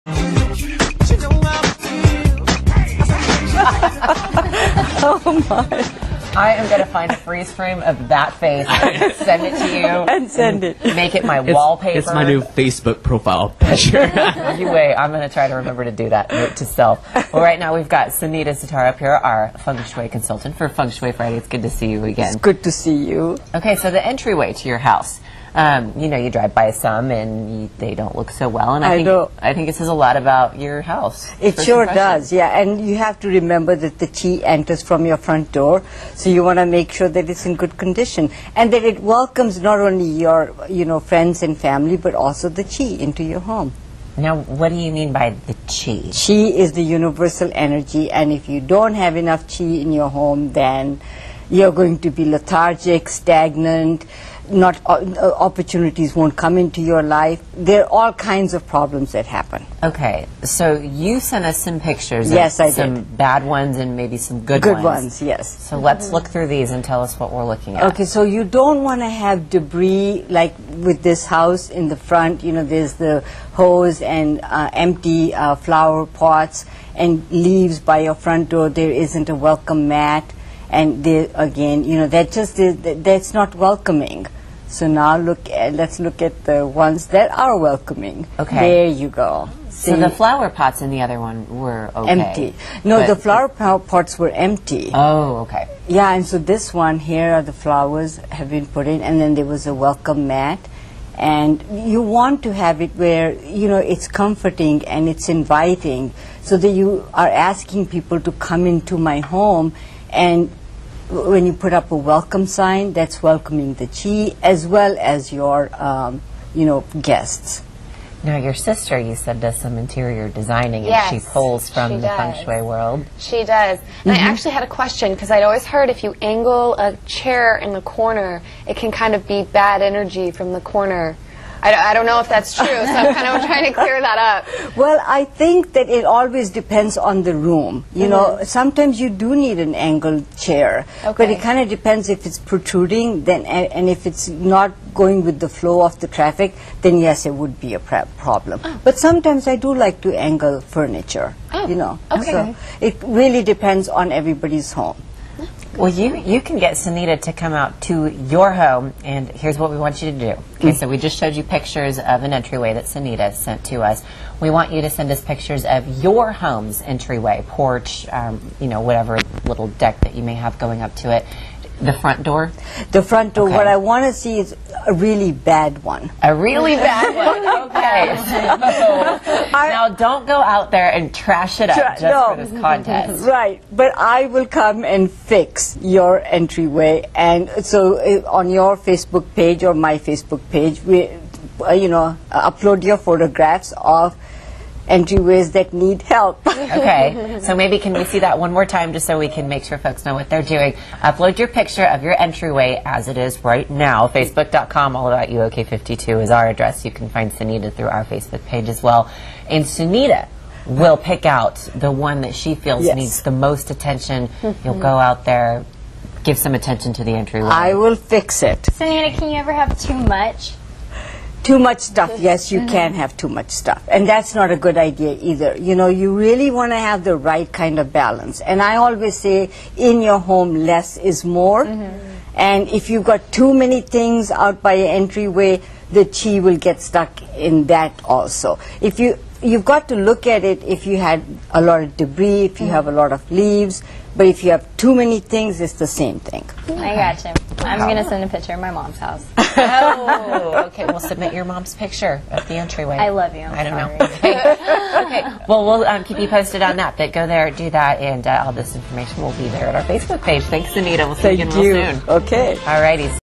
Feng Shu Friday: How to fix your entryway!
Sunita explains why it's important to keep the entry to your home in good condition!  Enter the contest to win your entryway makeover*!  (*For Oklahoma residents only!)